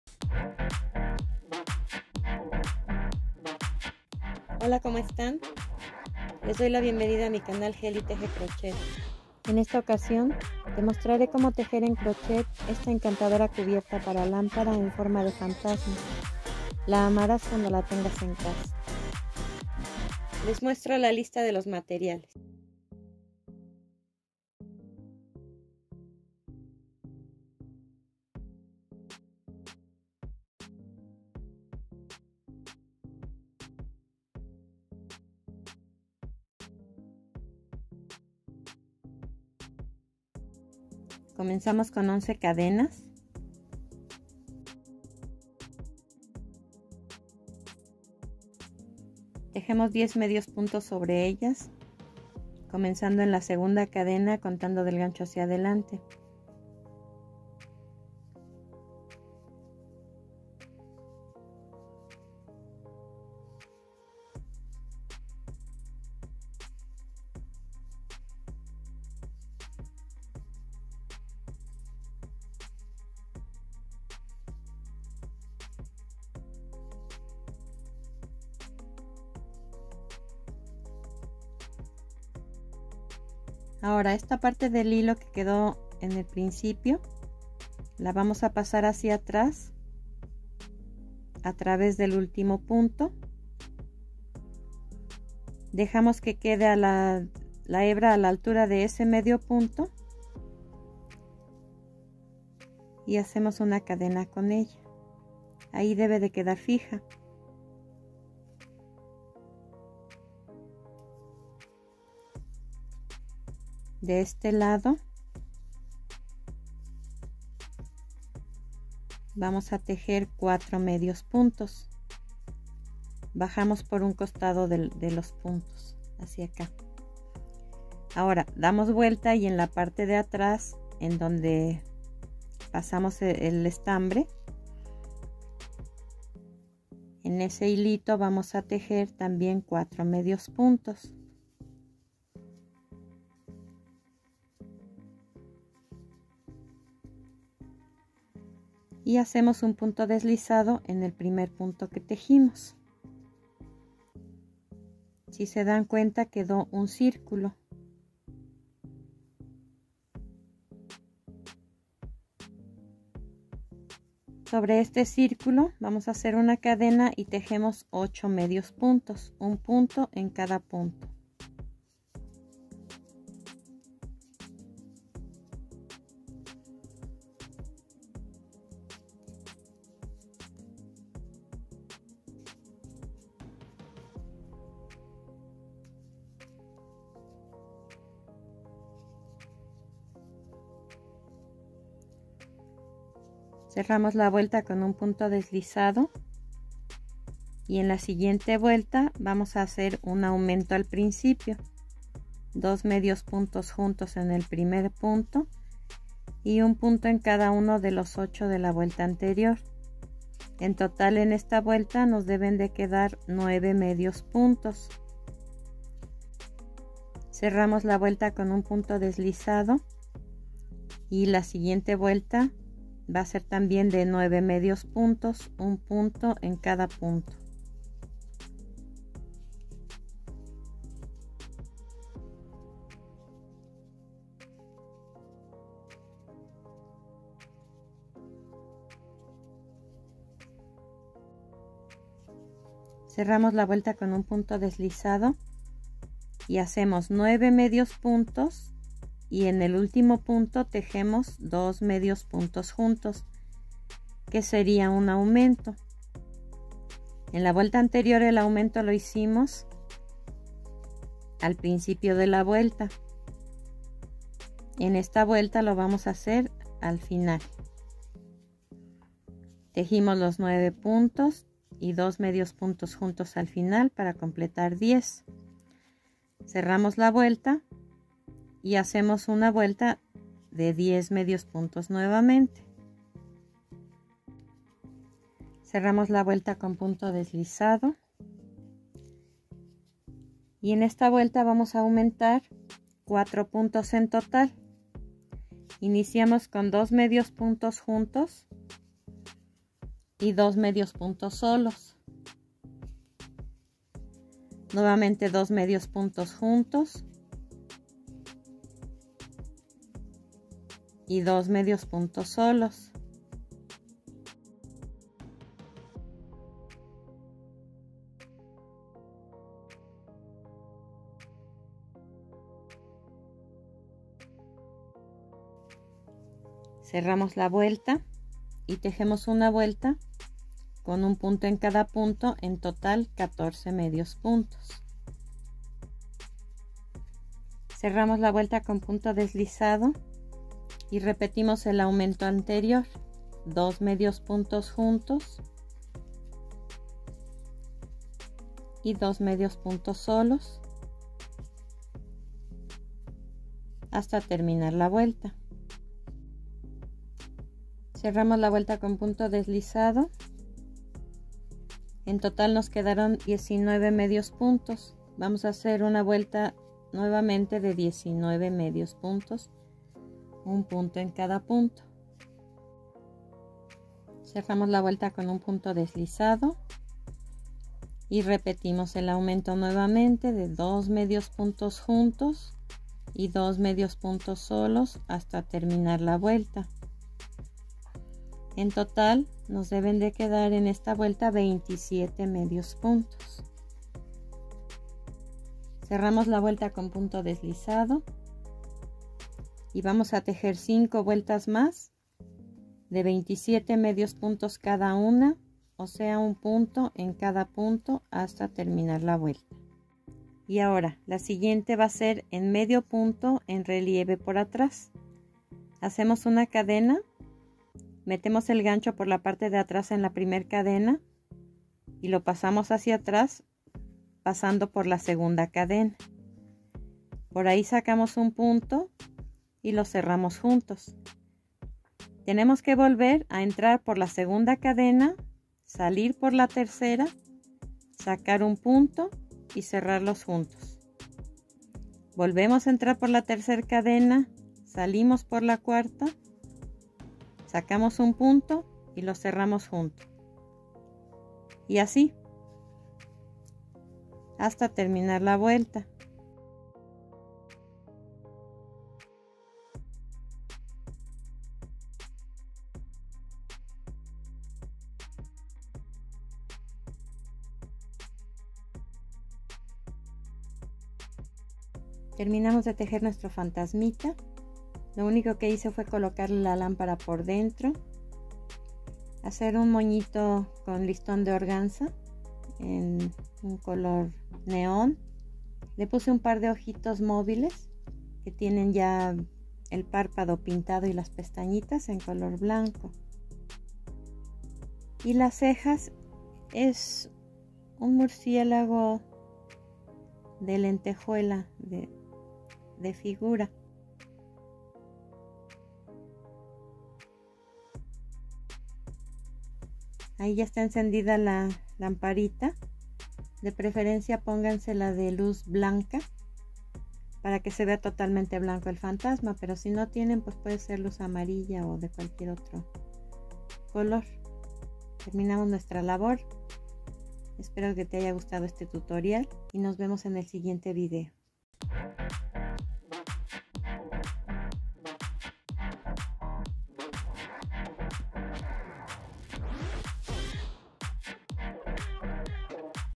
Cubierta para lámpara en forma de Fantasmita | CROCHET | Ghost Lamp
Cubierta para lámpara en forma de fantasma en crochet
W/Subtitle in English U.S.A.  🇺🇲

 CORREO ELECTRÓNICO
Cualquier duda o comentario estoy a sus órdenes